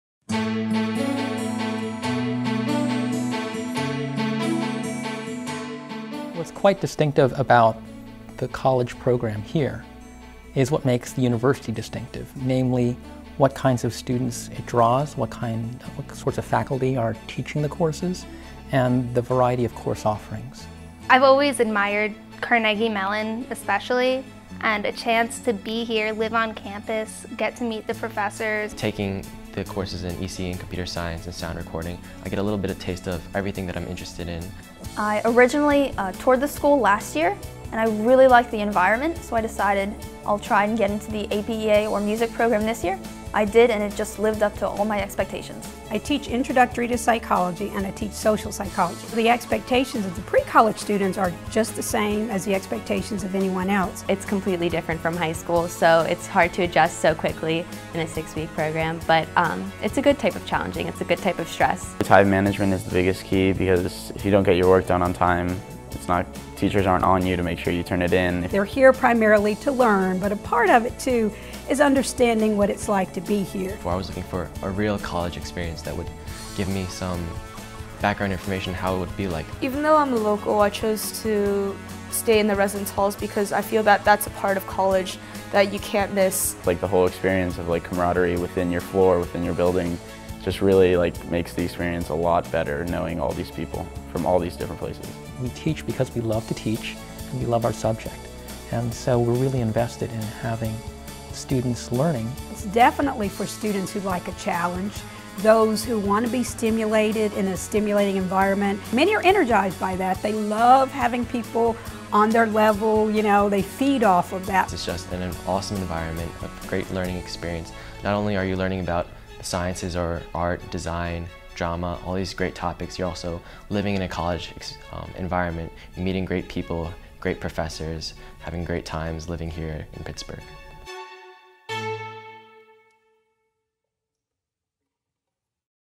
Pre-College APEA OLD
The APEA program provides talented, motivated high school students the opportunity to take university courses at Carnegie Mellon University during the summer. Students earn college credit while working in an academic environment mirroring that which the student would encounter during the first year of college.